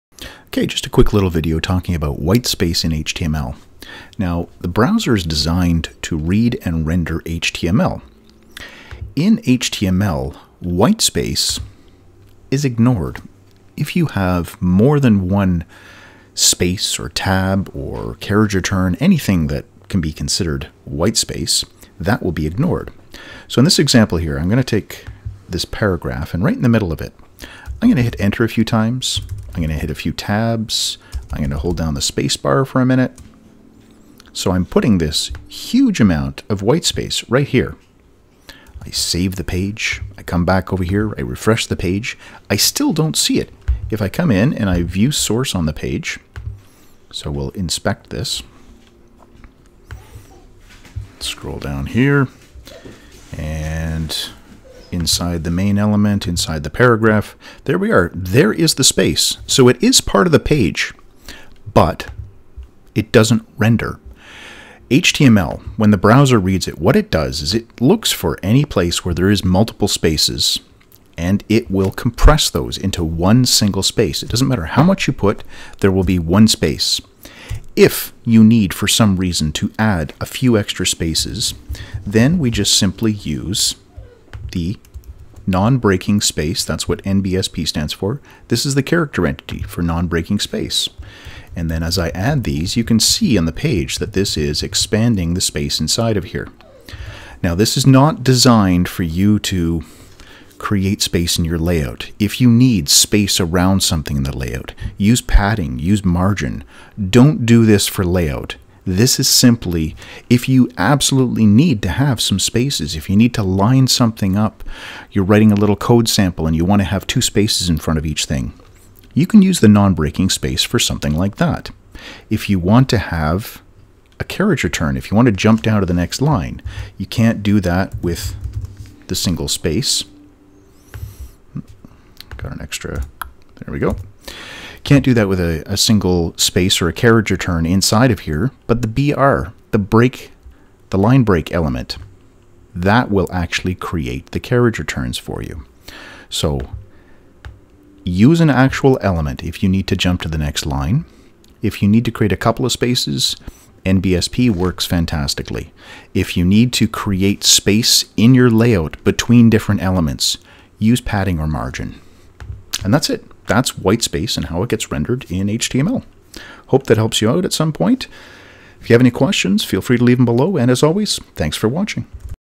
Whitespace in HTML
This video covers the appropriate use of Whitespace in your layout as well as how the browser reads the whitespace from inside your HTML files.